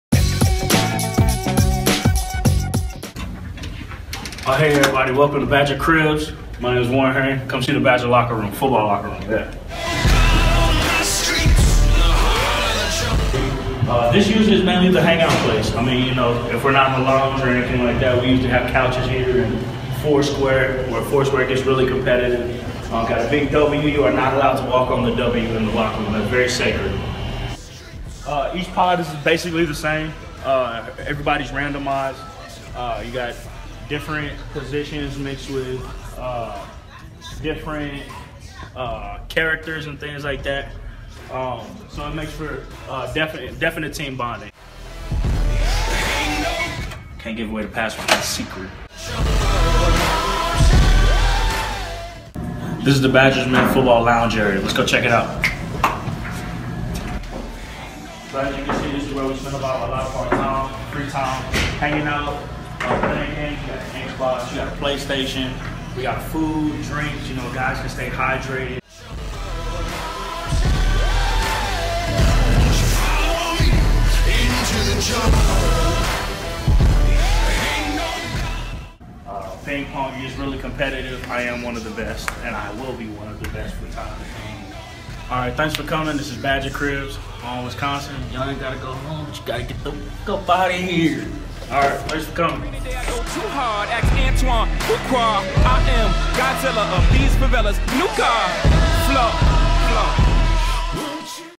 Badger Cribs: Camp Randall
An inside look at the UW-Madison football locker room at Camp Randall. Thanks to Warren Herring for giving us an exclusive tour of this amazing facility! Video made for J202 Final Project. If you like the footage visit Badger-to-Badger for more information about our university.